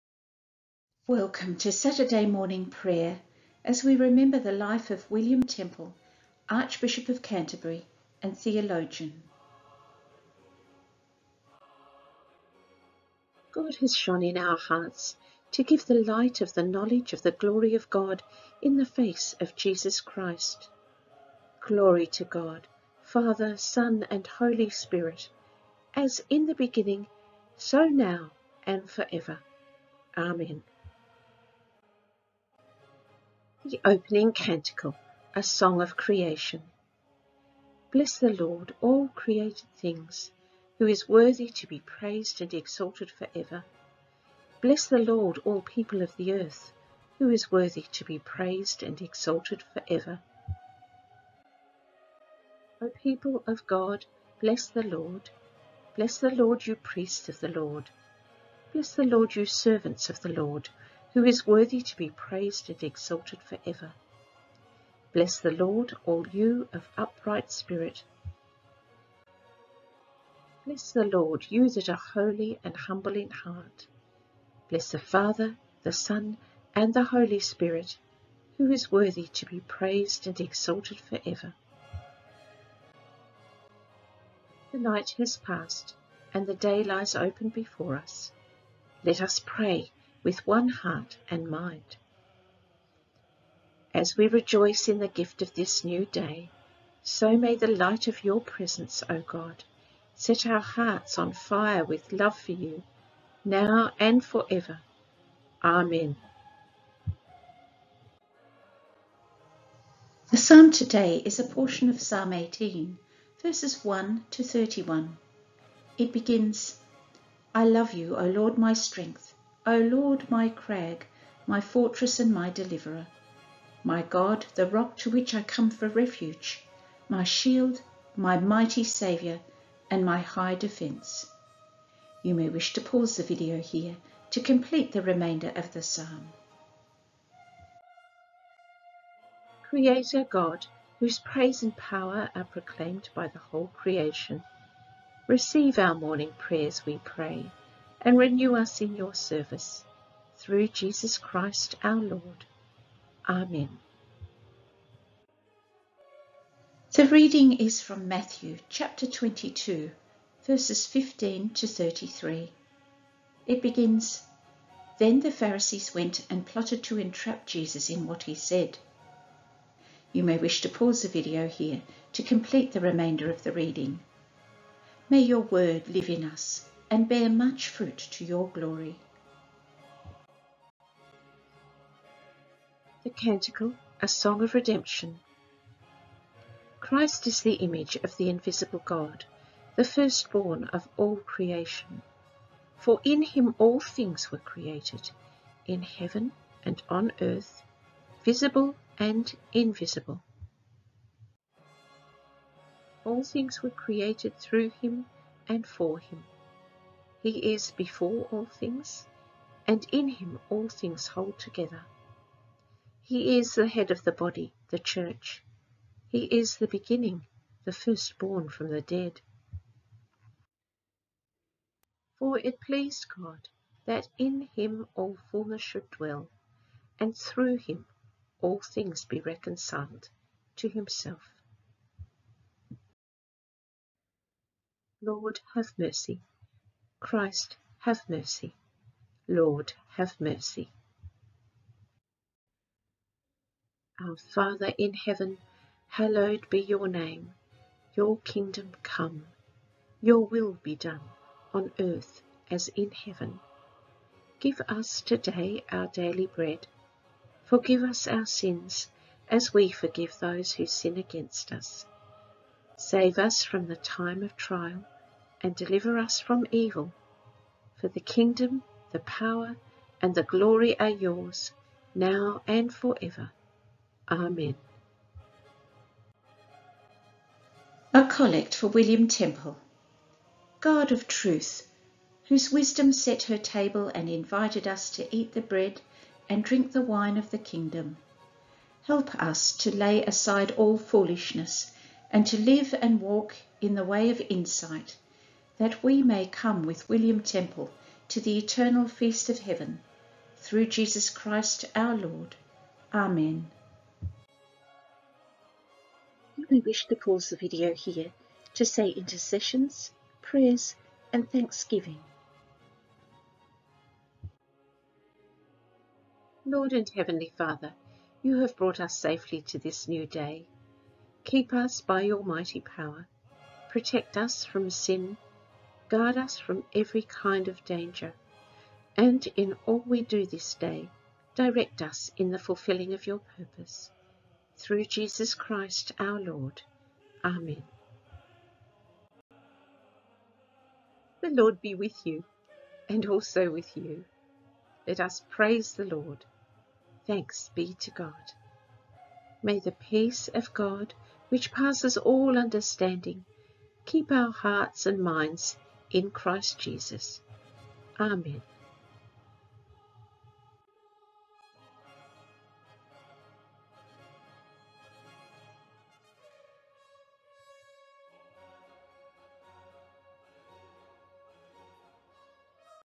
Saturday Morning Prayer 21 11 06
William Temple was born in 1881 and baptised on this day in Exeter Cathedral. His father was Bishop of Exeter and later Archbishop of Canterbury.
William excelled in academic studies and developed into a philosopher and theologian of significance.
He developed a remarkable ability to sum up an issue, expressing the pros and cons so clearly and fairly that the original opponents often ended up agreeing with each other.
This ability served him in good stead later when he moderated conferences on theological and social issues.
After ordination, he quickly made a mark in the Church and at forty became a bishop.
Within a decade he was Archbishop of York. He is especially remembered for his ecumenical efforts and also for his concern with social issues, contributing notably to the debate which led to the creation of state welfare provision after the Second World War. He died aged 63 in 1944, two years after his translation to the See of Canterbury.
When he died in 1944, England mourned “the people’s archbishop.” He was a light to England, and a light to the Church.
Temple's admirers have called him "a philosopher, theologian, social teacher, educational reformer, and the leader of the ecumenical movement of his generation," "the most significant Anglican churchman of the twentieth century," "the most renowned Primate in the Church of England since the English Reformation,"
But if he were here, he would remind us to think less on him, and turn toward Christ, the one true light. For while the darkness abides, it is we who must be the beacons—we who reflect the light of Christ in the world.
“The great question for everyone,” Temple wrote, “is whether we will ‘walk in darkness’ or ‘walk in light.’”
May you walk in light today and every day,
Rev’d Mandy

(Images are from Canterbury Cathedral)
Reading:
Psalm 18.1-31
Matthew 22.15-33